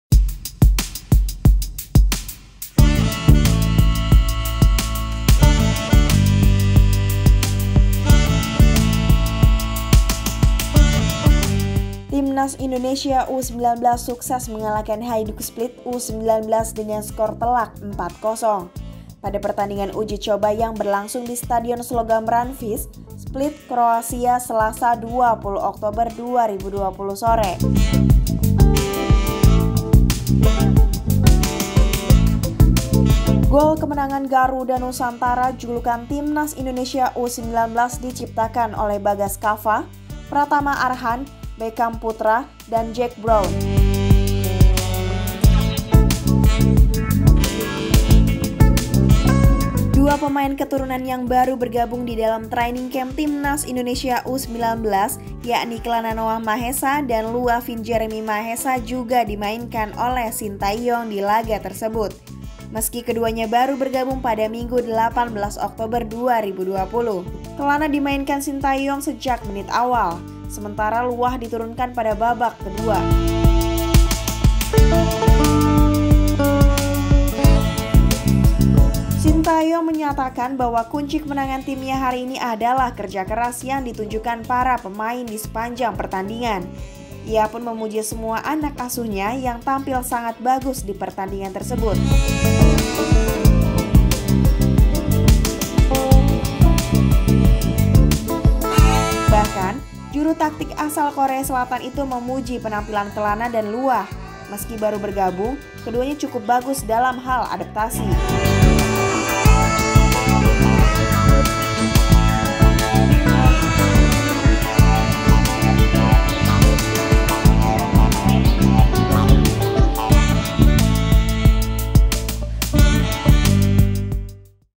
Timnas U-19 Cukur Hajduk Split 4-0
timnas u19,
hajduk split,
menang telak,

Timnas Indonesia U-19 sukses mengalahkan Hajduk Split U-19 dengan skor telak 4-0 pada pertandingan uji coba yang berlangsung di Stadion Sloga Mravince, Split, Kroasia, Selasa (20/10/2020) sore.